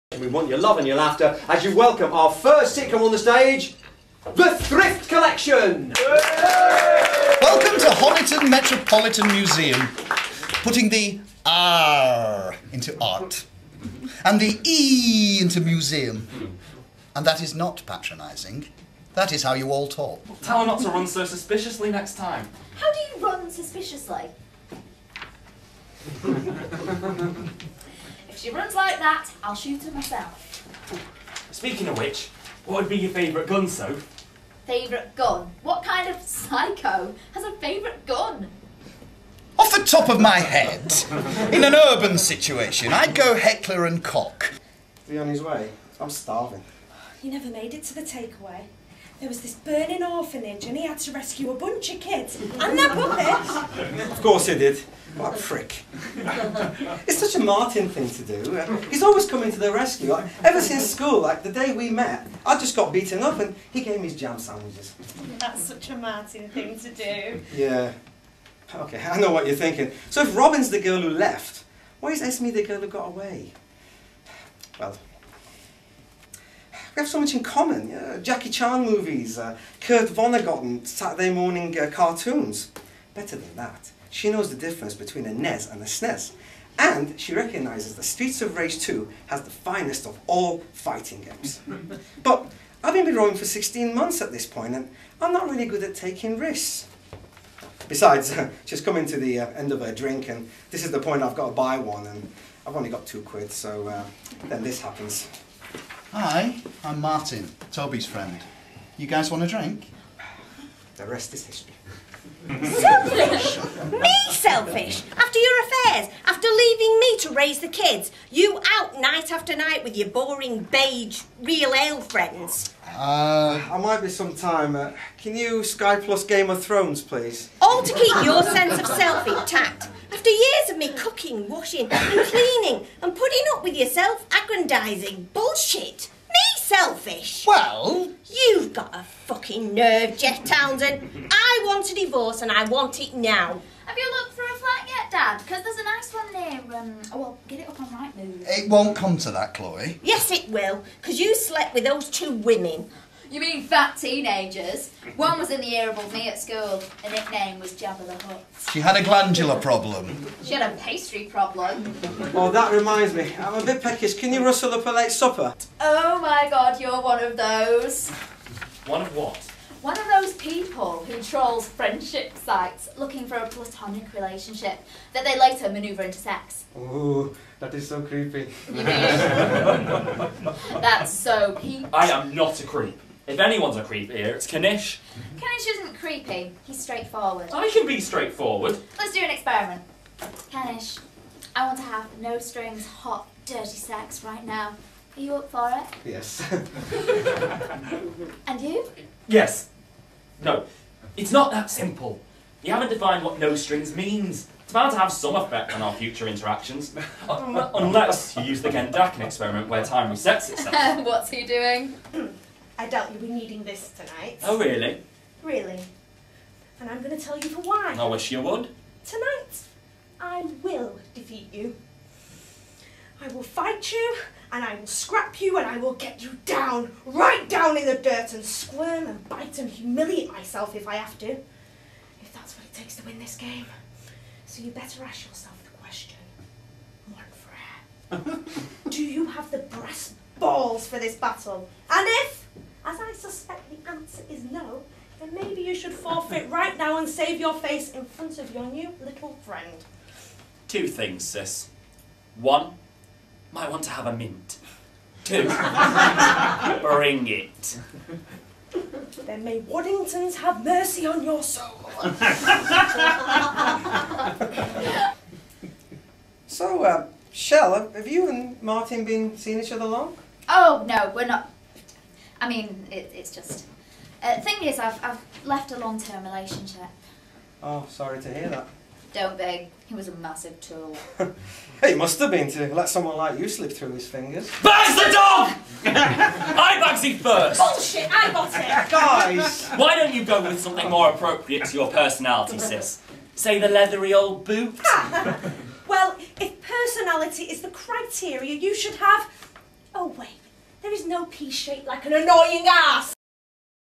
Sitcom Trials Manchester July 2012
The Manchester Sitcom Trials team, July 2012:  Jog Maher, Christine Dalby, Amir Rahimzadeh, Lisa Connor, Michael Loftus and actor-producer Michelle Ashton. Presenter Kev F just out of shot

The running order was:

Thrift Collection by Graeme & Nicky Knowles
My Best Mate by Bob Ferris
End Of The Line by Oliver Ley
Post Docs by Eoin Carney
Games Night by Ed Campbell